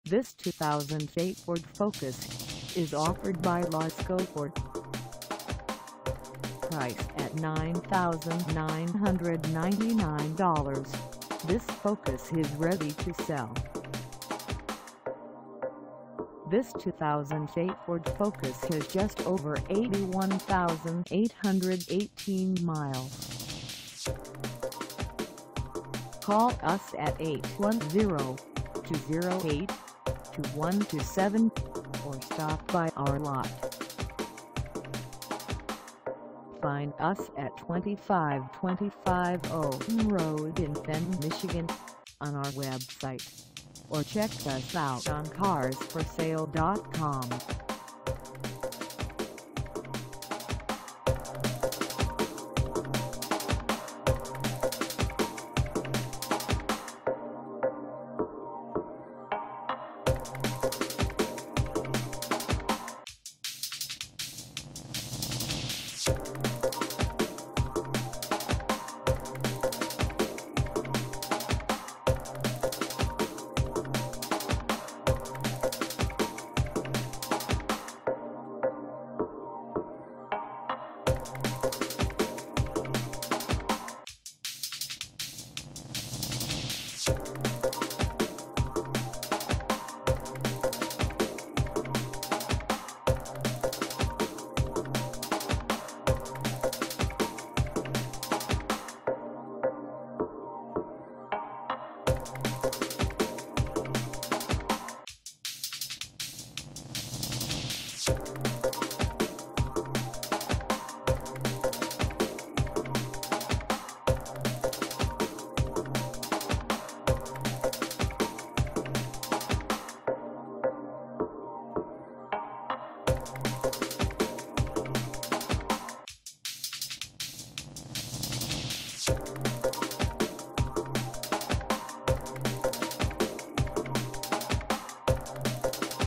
2008 Ford Focus - for sale in Fenton, MI 48430
LASCO FORD
2525 Owen Road

Come test drive this 2008 Ford Focus  for sale in Fenton, MI.

GREAT GAS MILEAGE!!! This spacious and sporty car has a roomy interior with comfortable seating for 5, a peppy 4 cylinder engine that's great on gas, a lot of safety features and convenience options!! Come on out today and take it for a ride!! During our Lasco Inspection we replaced both front sway links, and the engine mount! 3 Month/3,000 Mile Powertrain Limited Warranty. All Vehicles from 2004 and newer. Vehicles with 80,000 to 125,000 Miles.$200 Deductible.